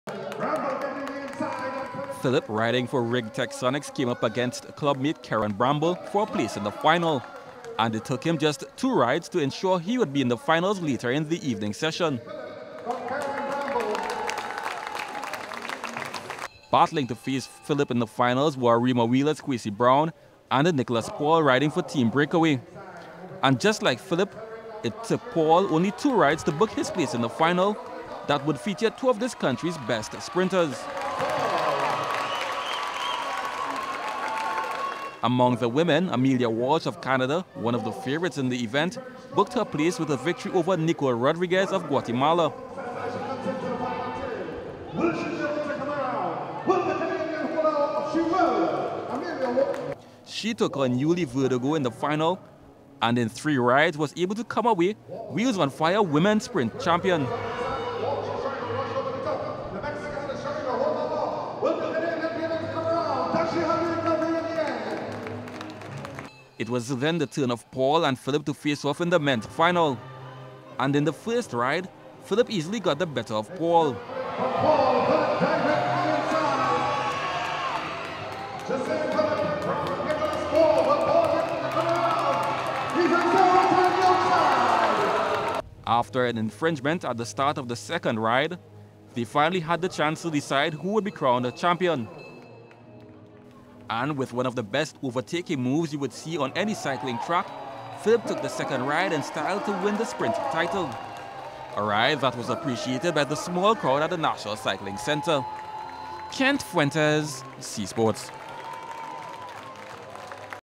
Njisane Phillip Crowned Sprint Champion At ‘Fire on Wheels’
Njisane Phillip was crowned sprint champion on Saturday night at the PSL Fire on Wheels track cycling competition down at the National Cycling Velodrome in Couva.

Phillip had to get the better of Nicholas Paul in a sensational finale.